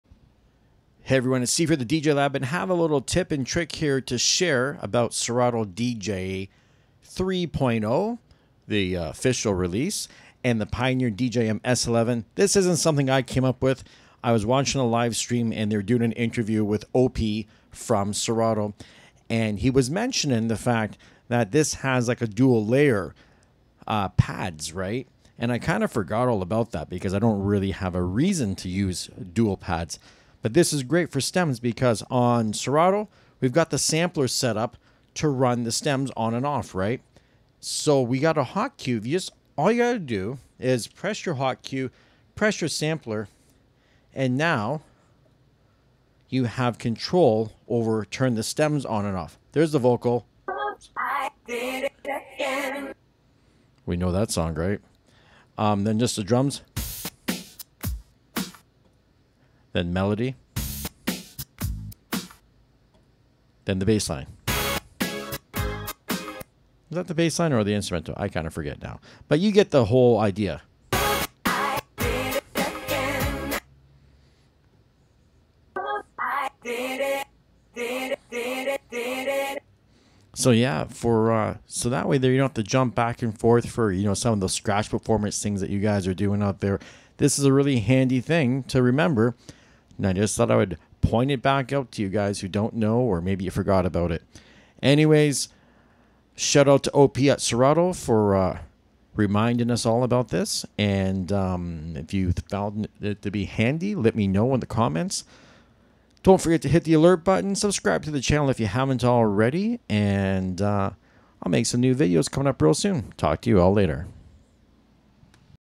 Serato DJ Pro 3.0 Tips & Trcks - Hot Cues & STEMS
In this video I''ll show you how to have access to 4 hot cues and your STEMS on Pads with the Pioneer DJM-S11